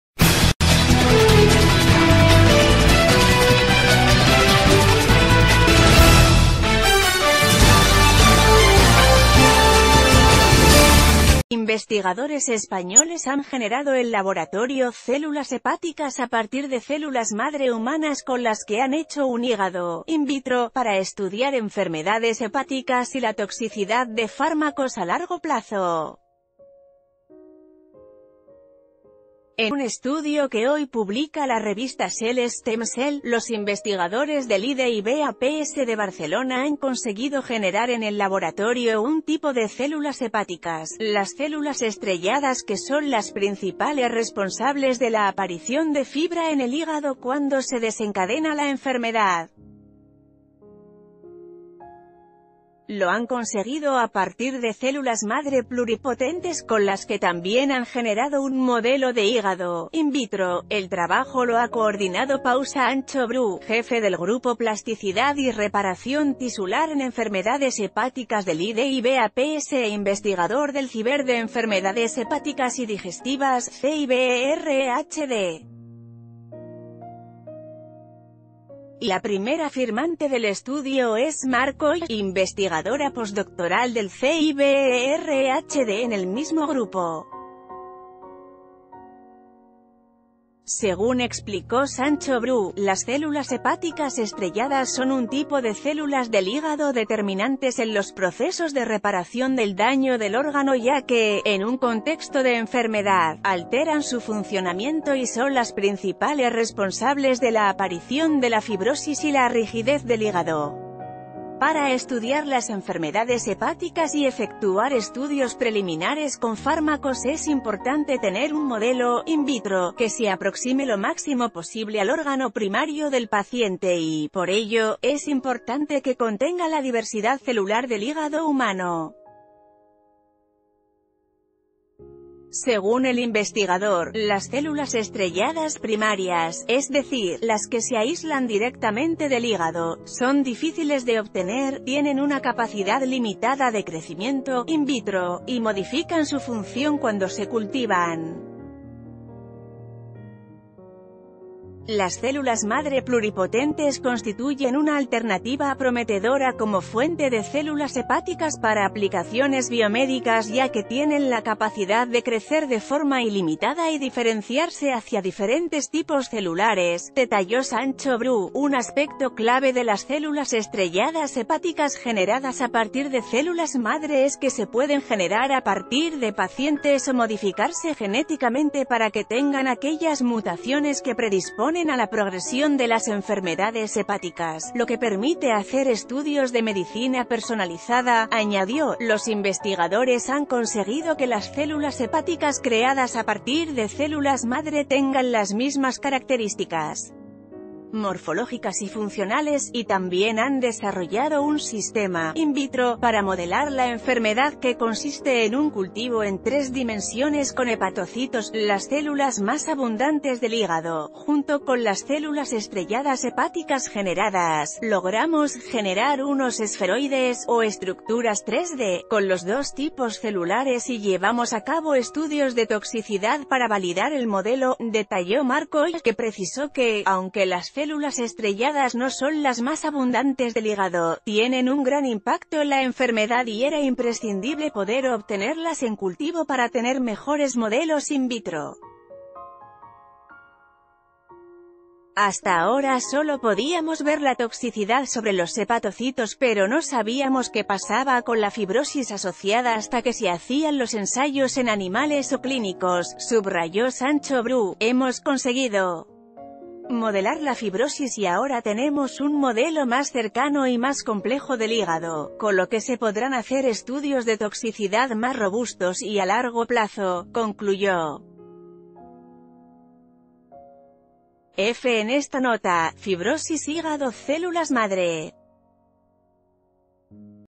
Crean hígado con células madre para fines investigativos
Investigadores españoles han generado en laboratorio células hepáticas a partir de células madre humanas con las que han hecho un hígado 'in vitro' para estudiar enfermedades hepáticas y la toxicidad de fármacos a largo plazo. En un estudio que hoy publica la revista Cell Stem Cell, los investigador...